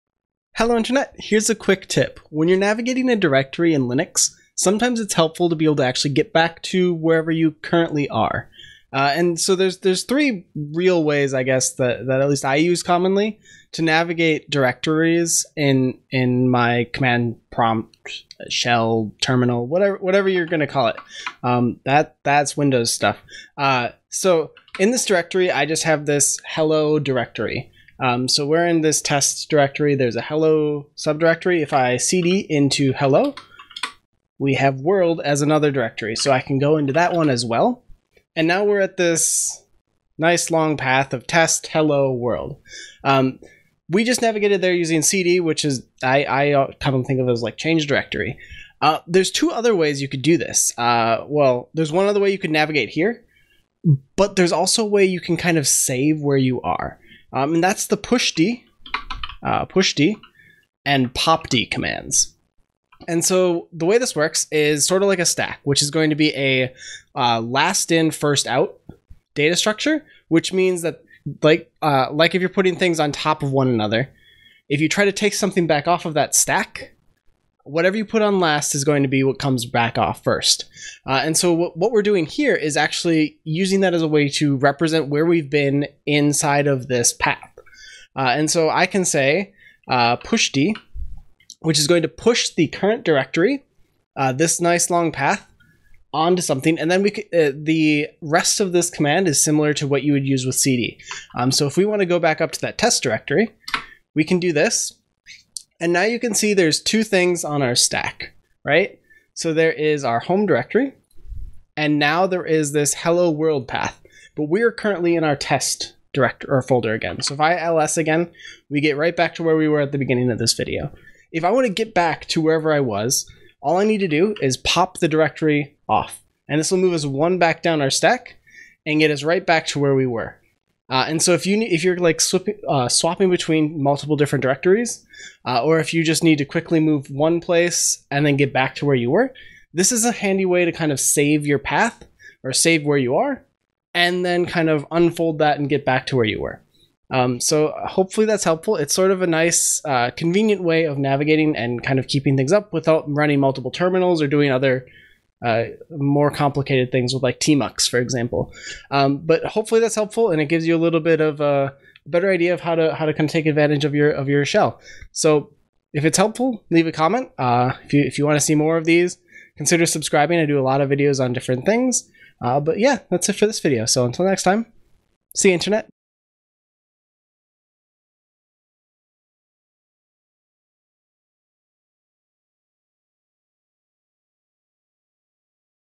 Navigate Your Shell With pushd and popd - Quick Tip
There are a few ways to navigate your shell in terminals. One of the less common but useful ways I've found when working with multiple directories takes advantage of the directory stack most terminals maintain. This is a stack which contains a series of directories in the order they were visited. We can interact with this stack using `pushd` and `popd` to push new directories onto the stack or pop directories off of the stack respectively. This allows you to create a history of previous places you've been in your terminal session and navigate through them.

These commands are available in many unix shells (I use bash on Ubuntu in this video) as well as CMD and Powershell in Windows.